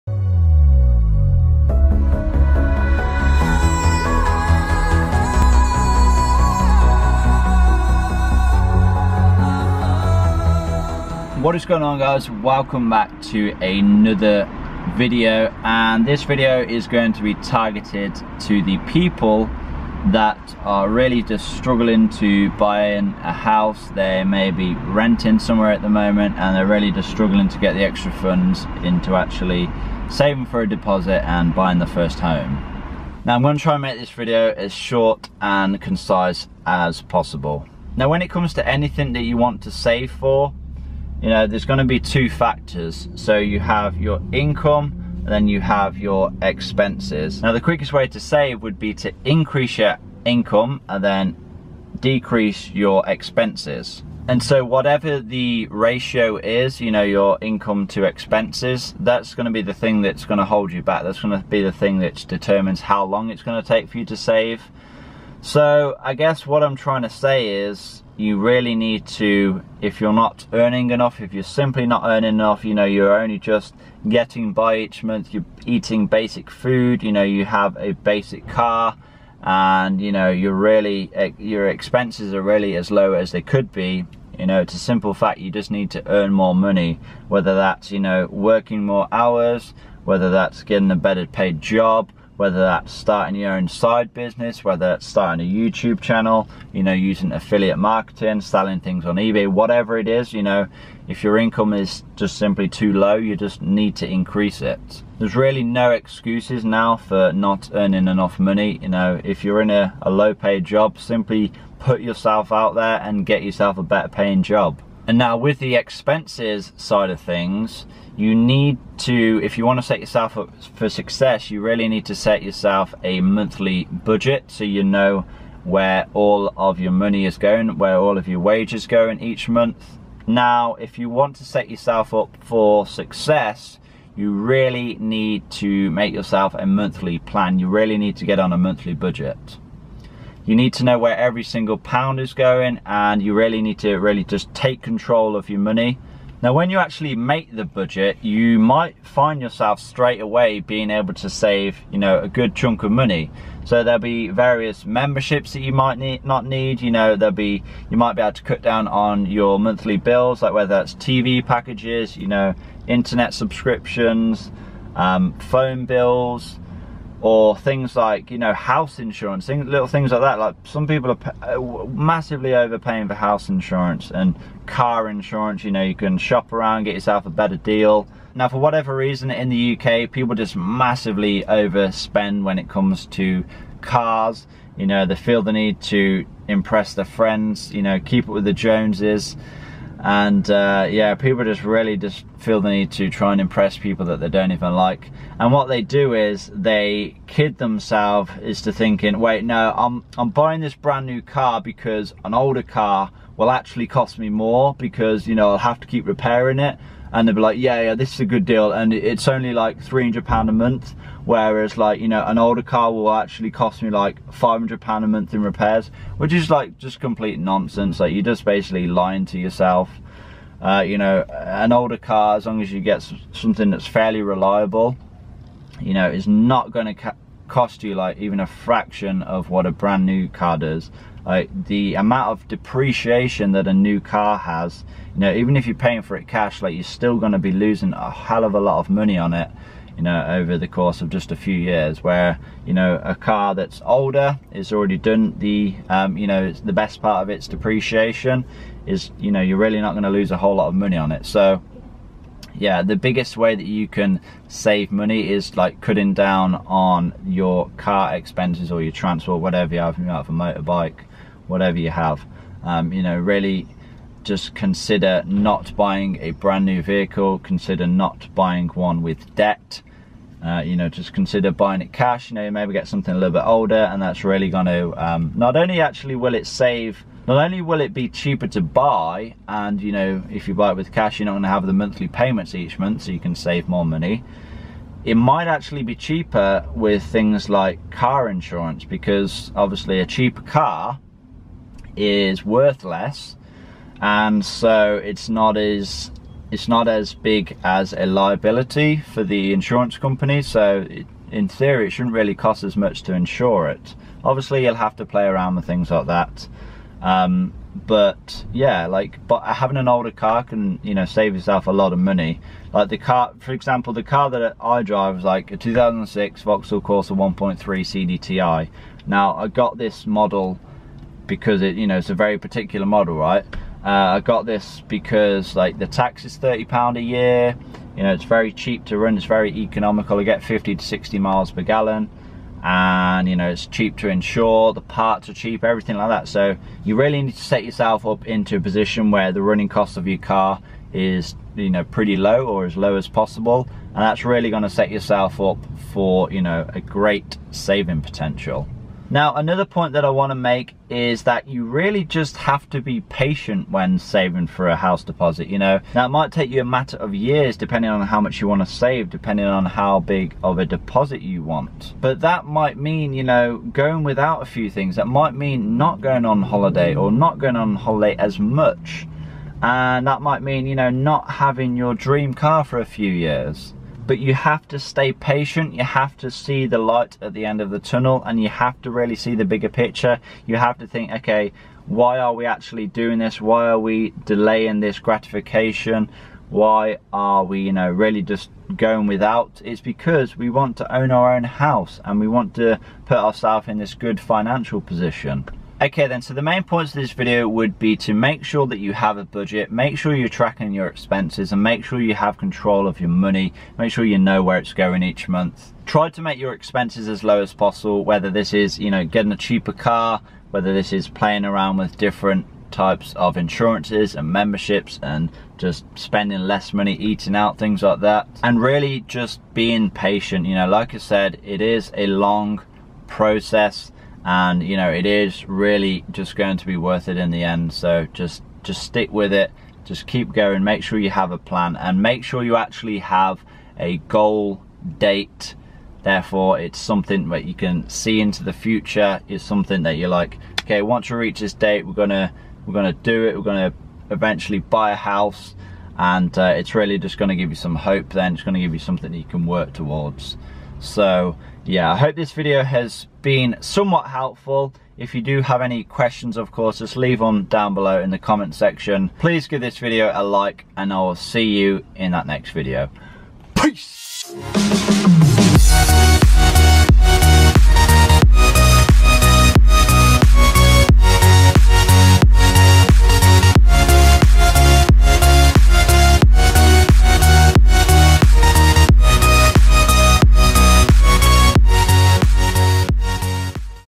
How To Save For a House Deposit When You Are Renting
In this video I talk about how it is more than possible to save for a house deposit while still renting your own place.